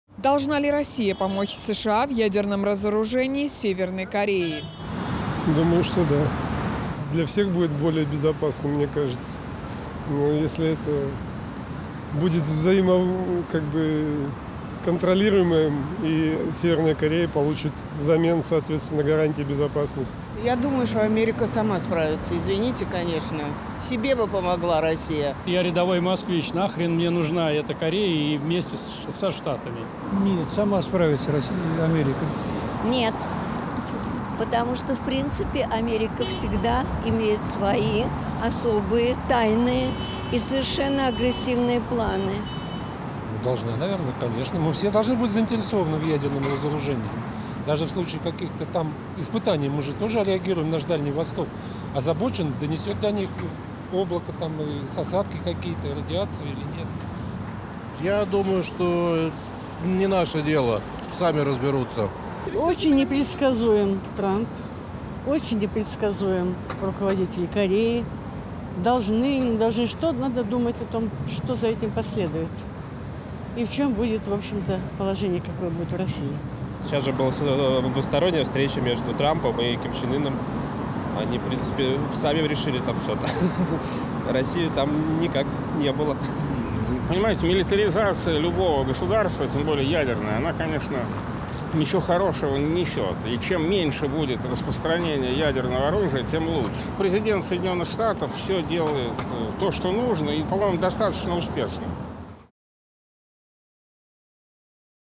Должна ли Россия помочь США в ядерном разоружении Северной Кореи?
Опрос прохожих на улицах Москвы